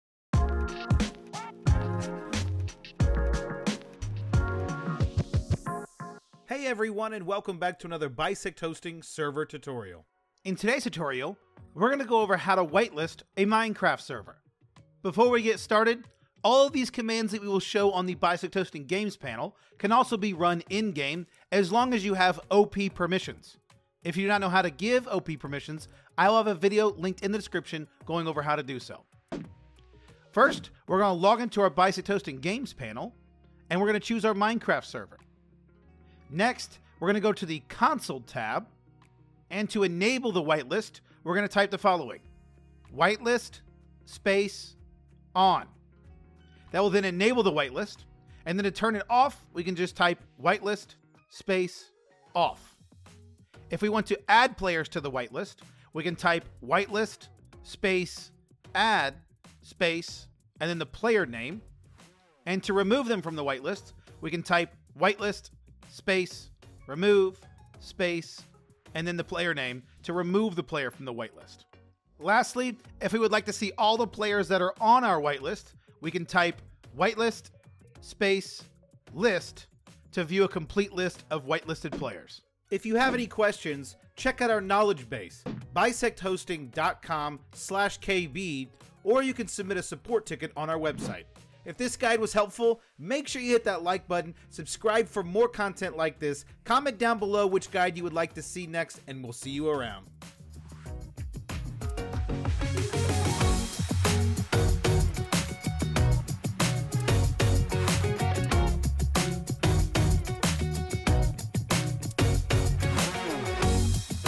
How to Whitelist a Minecraft Server!
Learn How to Whitelist a Minecraft Server!

Whitelisting a server keeps unwanted players from joining the server without being added to the list. If you want players to join the server, adding them to this whitelist will allow them access.

TIMESTAMPS:

0:00 Intro
0:30 Turn on Whitelist
0:47 Turn Off
0:53 Add/Remove Players
1:14 List Players
1:22 Outro